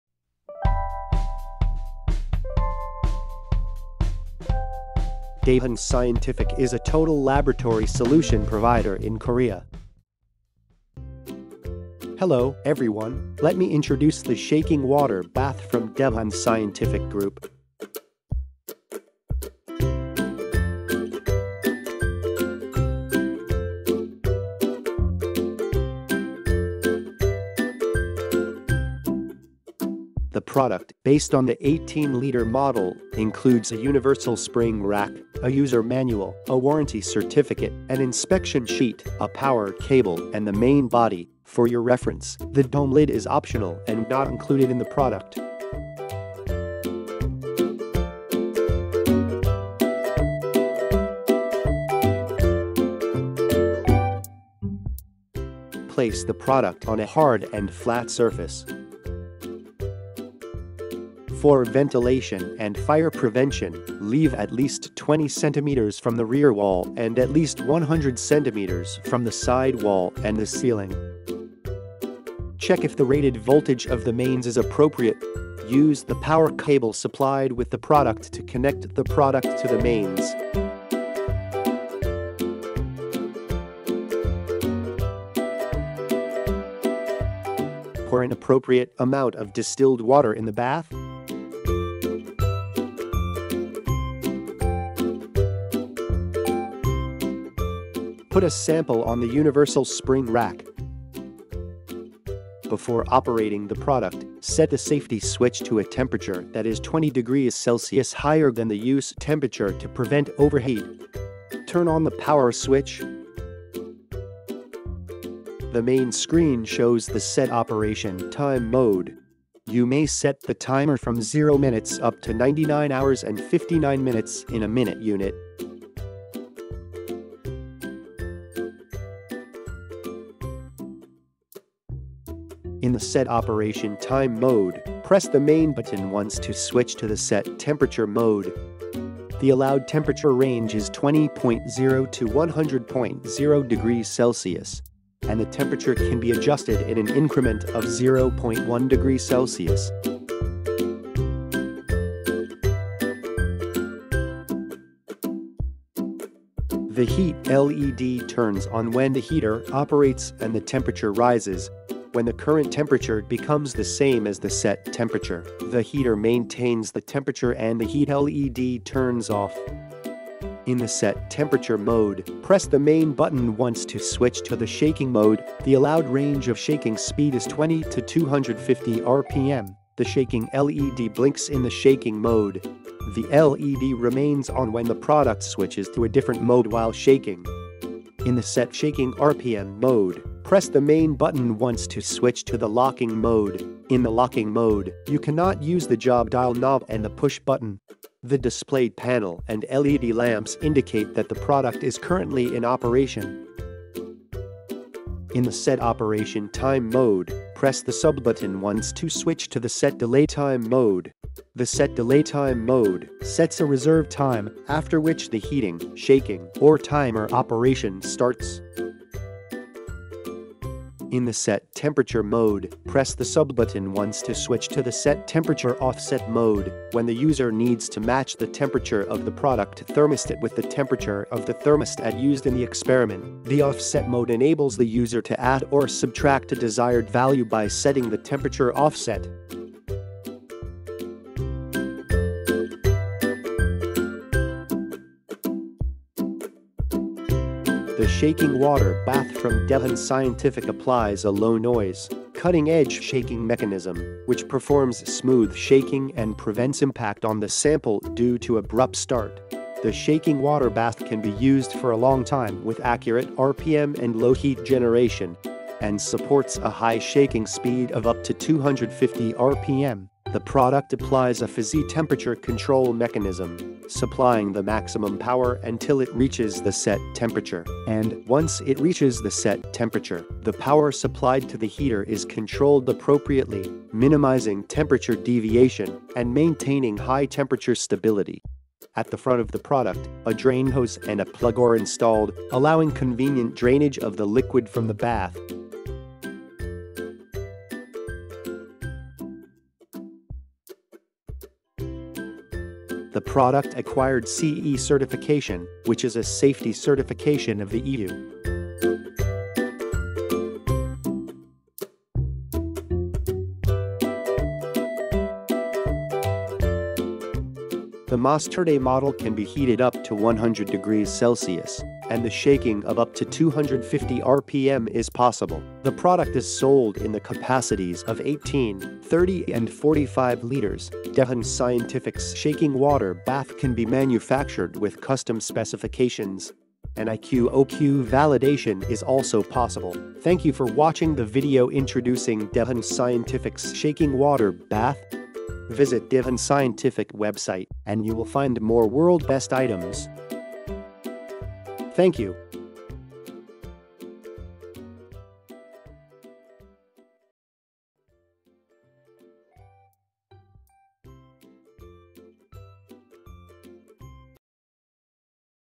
[DAIHAN Scientific] DAIHAN® Digital Precise Shaking Water Bath "MaXturdy TM" (with 네이버 클로바더빙) ENG
DAIHAN® Digital Precise Shaking Water Bath “MaXturdy TM ” , Reciprocating Motion, 18·30·45 LitWith Universal Spring Rack, Digital Fuzzy Control, up to 2 Lit Flask, 20~250 rpm, up to 100℃, ±0.1℃, with Certi. & Traceability정밀 진탕 항온수조, 왕복 진탕 운동, 디지털 피드백 제어, 고성능 쉐이킹 엔진 탑재